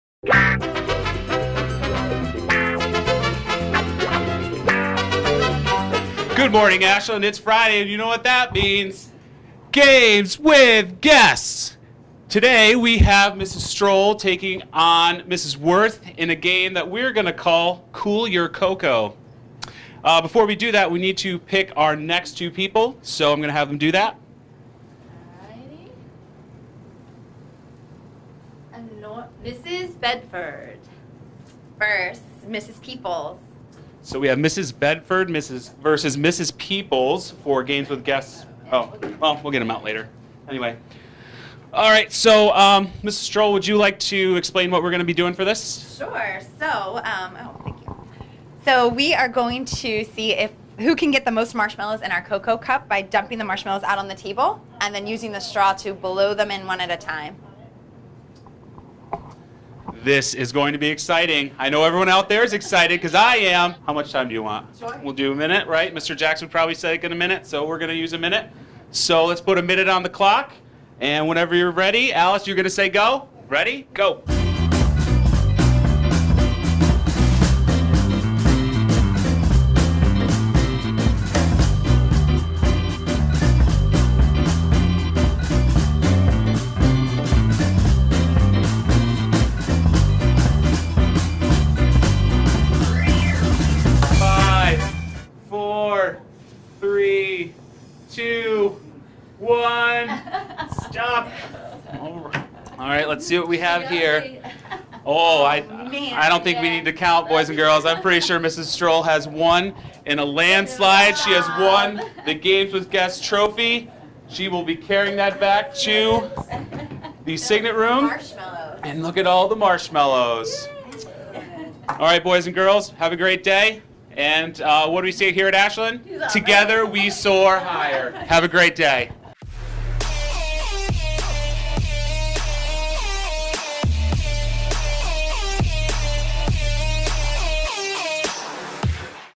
2019-1-25 Games with Guests Cool Your Coco
This week on Games with Guests Ms. Strole and Mrs. Wirth try to "Cool Your CoCo" by blowing marshmallows into their cups. This was a lot more difficult than it sounds.